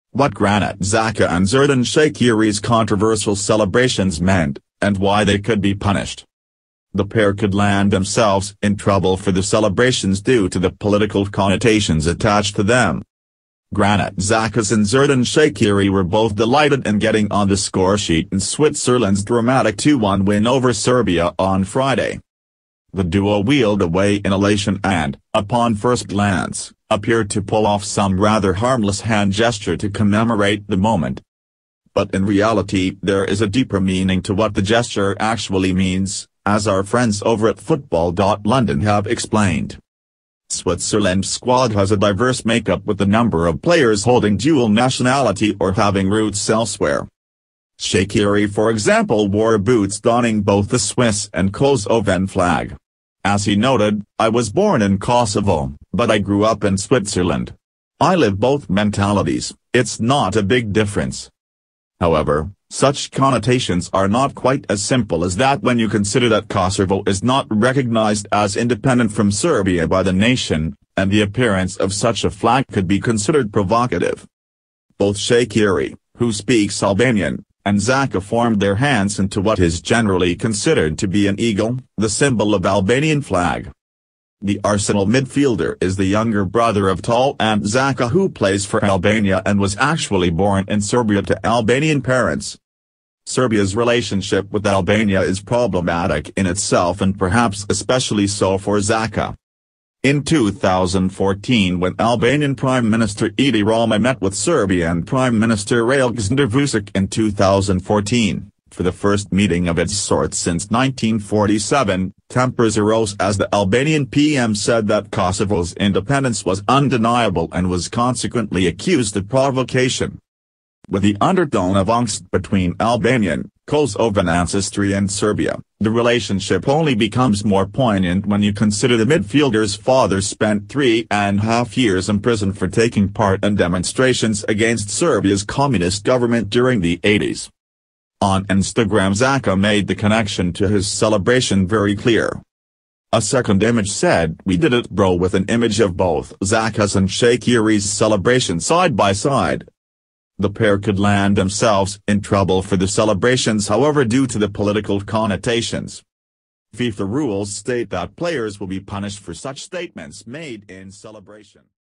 What Granit Xhaka and Xherdan Shaqiri's controversial celebrations meant
What Granit Xhaka and Xherdan Shaqiri's controversial celebrations meant - and why they could be punished
The pair could land themselves in trouble for the celebrations due to the political connotations attached to them
Granit Xhaka's and Xherdan Shaqiri were both delighted in getting on the score sheet in Switzerland's dramatic 2-1 win over Serbia on Friday.
The duo wheeled away in elation and, upon first glance, appeared to pull off some rather harmless hand gesture to commemorate the moment.
But in reality there is a deeper meaning to what the gesture actually means, as our friends over at football.london have explained.
Switzerland's squad has a diverse make-up with a number of players holding dual nationality or having roots elsewhere.
Shaqiri for example wore boots donning both the Swiss and Kosovan flag. As he noted: "I was born in Kosovo, but I grew up in Switzerland. I live both mentalities, it's not a big difference."
However, such connotations are not quite as simple as that when you consider that Kosovo is not recognised as independent from Serbia by the nation, and the appearance of such a flag could be considered provocative.
Both Shaqiri - who speaks Albanian - and Xhaka formed their hands into what is generally considered to be an eagle - the symbol of Albanian flag.
The Arsenal midfielder is the younger brother of Taulant Xhaka who plays for Albania and was actually born in Serbia to Albanian parents.
Serbia's relationship with Albania is problematic in itself and perhaps especially so for Xhaka.
In 2014 when Albanian Prime Minister Edi Rama met with Serbian Prime Minister Aleksandar Vucic in 2014, for the first meeting of it's sorts since 1947, tempers arose as the Albanian PM said that Kosovo's independence was 'undeniable' and was consequently accused of provocation.
With the undertone of angst between Albanian/Kosovan ancestry and Serbia, the relationship only becomes more poignant when you consider the midfielder's father spent three and half years imprisoned for taking part in demonstrations against Serbia's communist government during the 80s.
On Instagram Xhaka made the connection to his celebration very clear.
A second image said 'We did it bro' with an image of both Xhaka's and Shaqiri's celebration side by side.
The pair could land themselves in trouble for the celebrations however due to the political connotations.
FIFA rules state that players will be punished for such statements made in celebration.